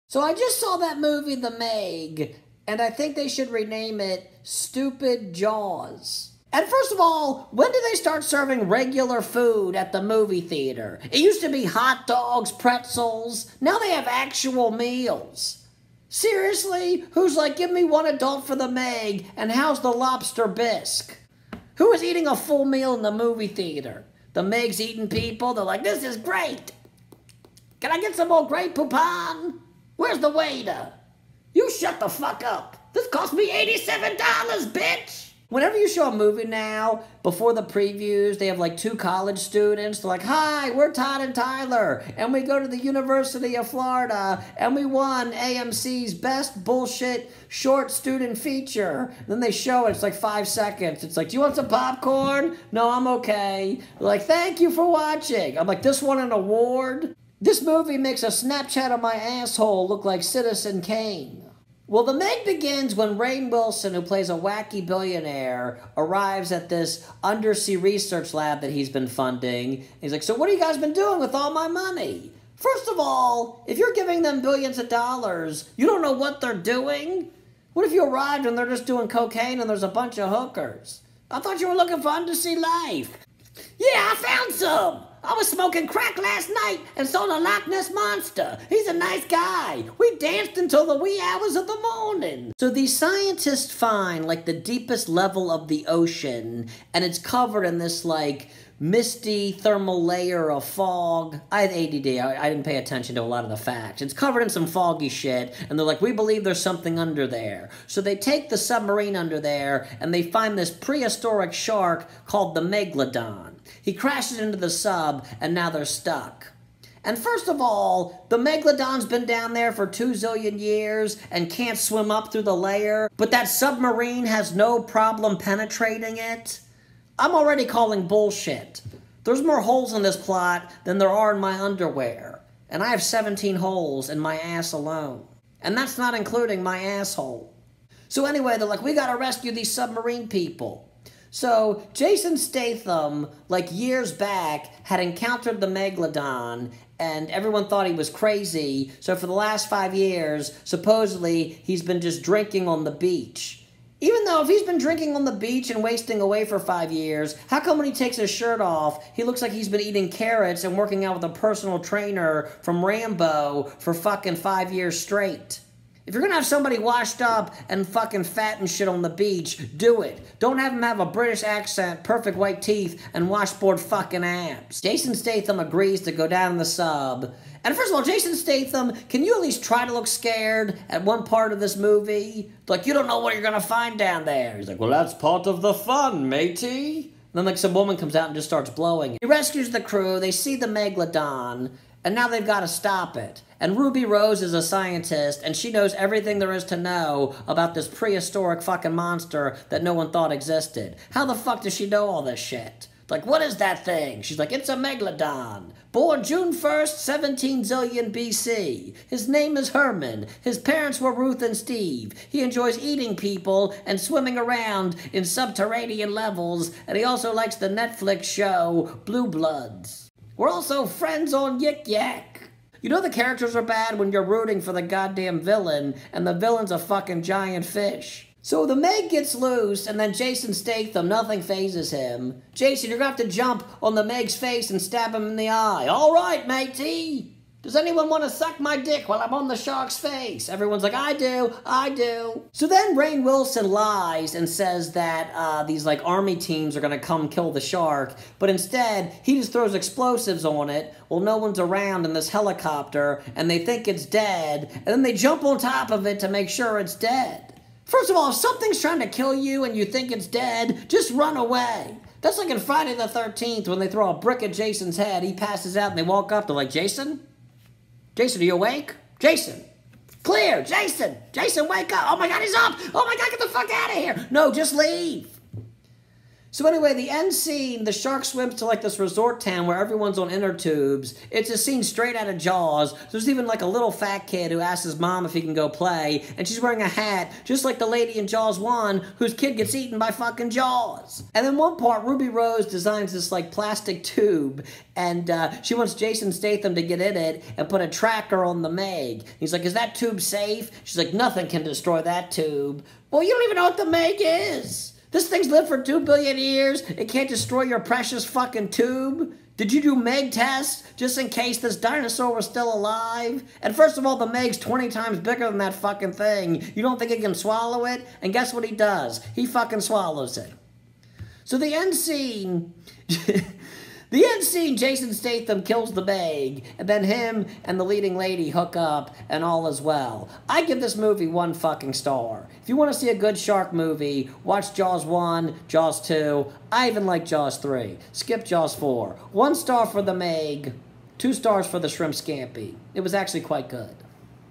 MY MOVIE REVIEW OF THE MEG
“SPOILER ALERT” EPISODE #2 I review the new movie “The Meg”.